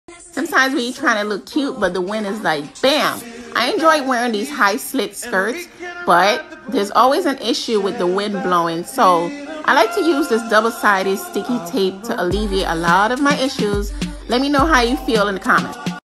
STYLE HACK: USE DOUBLE-SIDED STICKY TAPE
Use double-sided sticky tape to alleviate a lot of style issues...especially when wearing wrap skirts with high slits.